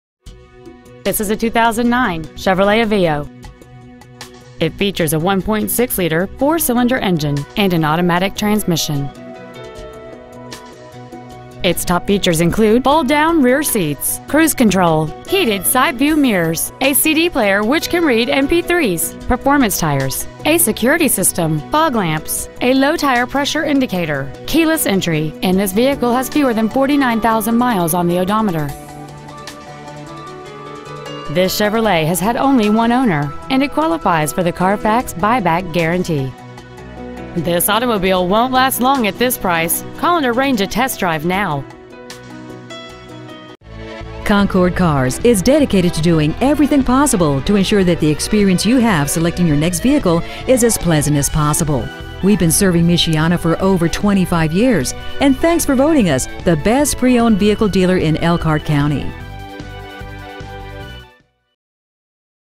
2009 CHEVROLET AVEO Elkhart IN Click here for price : http:/
We've been honored to serve the Elkhart IN area , we promise that your experience at our dealership will exceed your expectations !

 Year : 2009
 Make : CHEVROLET
 Model : AVEO
 Engine : L4 1.6L
 Trans . : AUTOMATIC
 Exterior : SPORT RED
 Miles : 48,689

 Stock : 389475

 2009 CHEVROLET AVEO Elkhart IN